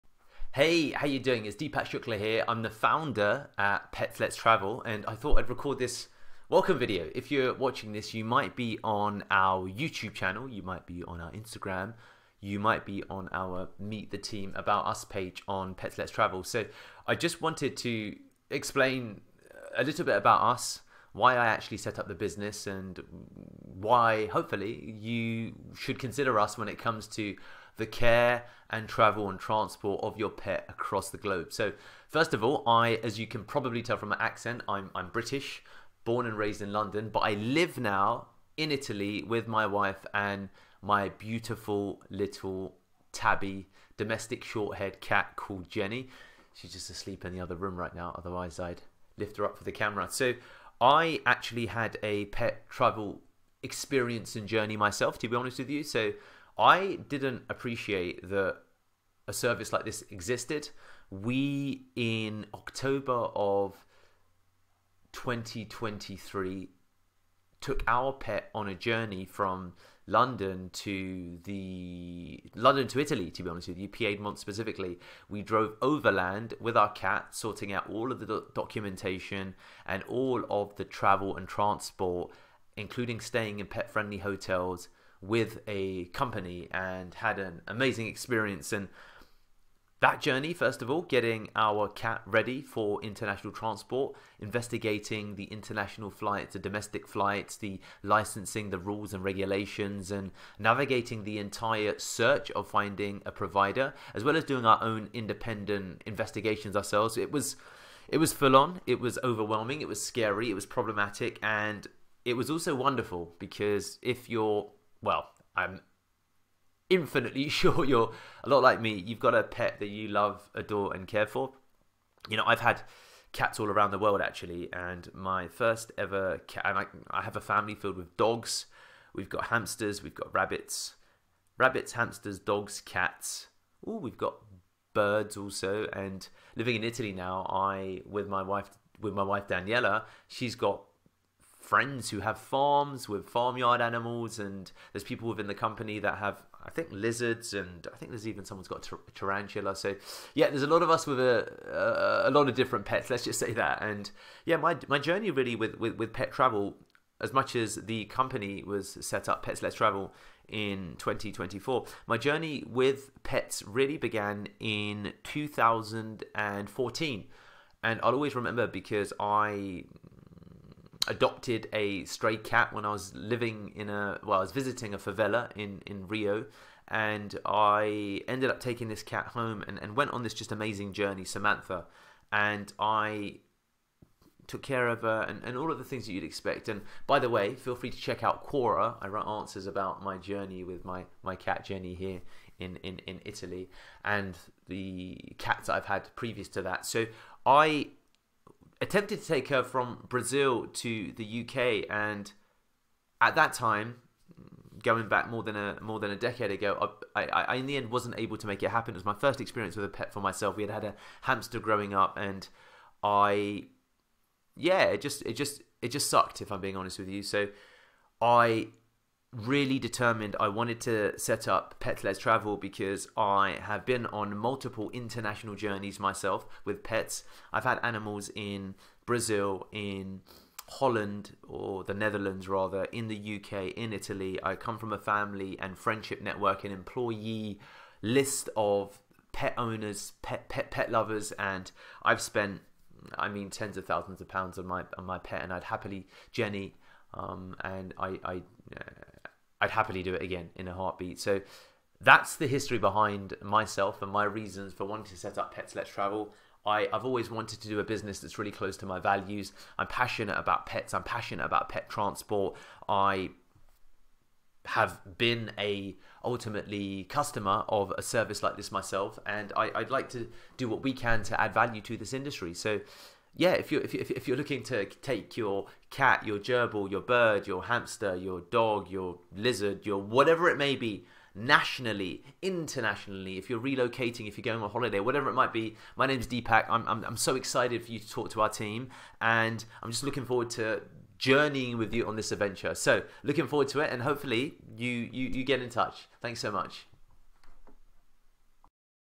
The Pets Let's Travel Story - Meet The Founder - Deepak Shukla
Meet the Founder of Pets Let's Travel - Deepak Shukla
,as he shares his personal journey and the inspiration behind the company. Learn about Deepak's passion for pets, his experience in pet travel, and his vision for providing top-notch care and transport services for pets worldwide. If you're a pet owner looking for reliable and compassionate pet transport solutions, get to know Deepak and the Pets Let's Travel team in this insightful video.

📚 CHAPTERS :

00:00 Pet travel experience from London to Italy.
03:37 Passionate about pet travel, dedicated to values.

It’s not a surprise when you consider our founder Deepak Shukla is an ex-Deloitte consultant and 30x marathon runner. 🏃🏻